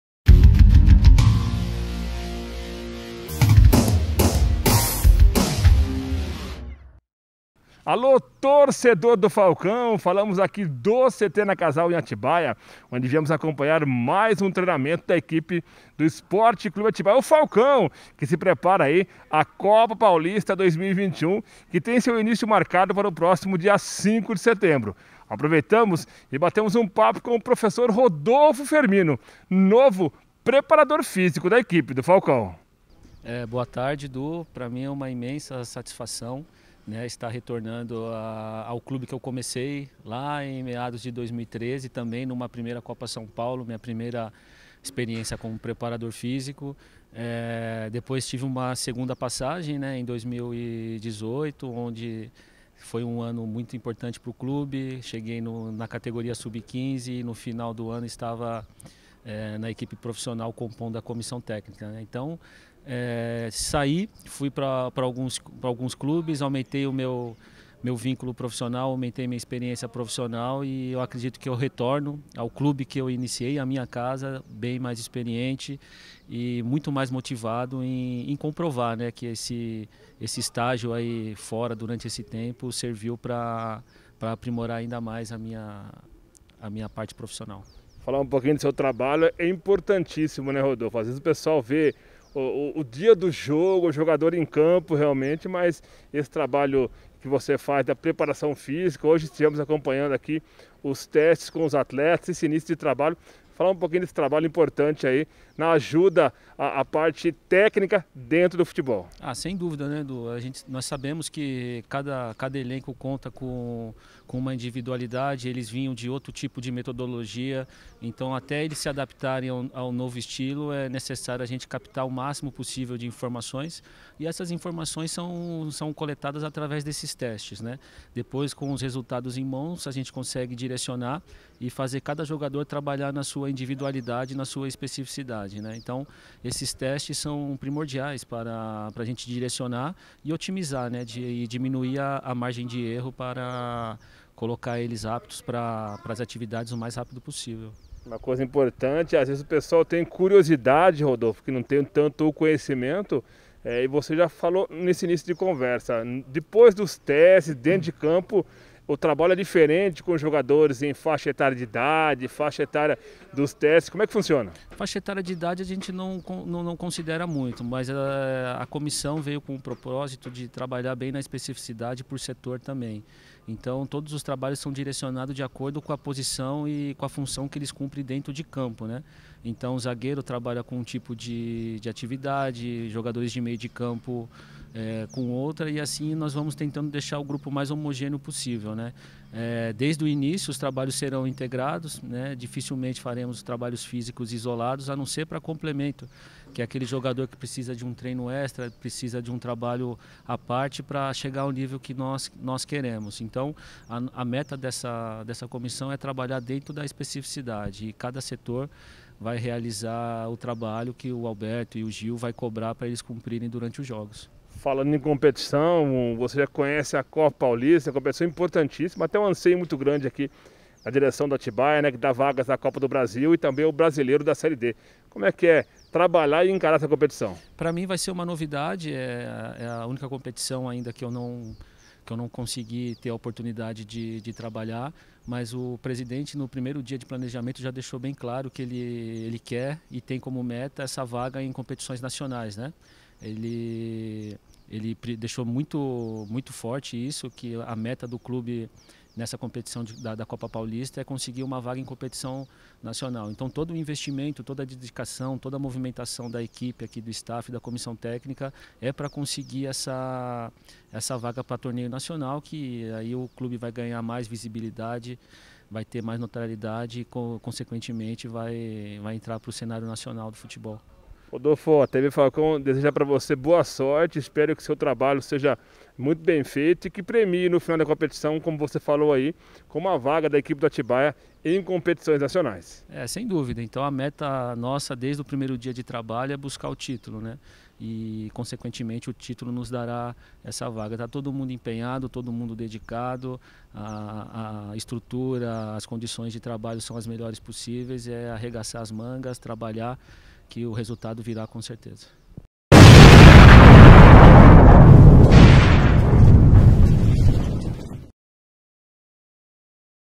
RODOLFO FERMINO - NOVO PREP.FÍSICO DO FALCÃO
A TV-Falcão acompanhou os trabalhos de testes físicos no CT-Nakazawa na cidade de Atibaia-SP e aproveitou para bater um papo com o PROF.RODOLFO FERMINO, novo preparador físico da equipe do Sport Club Atibaia, confiram !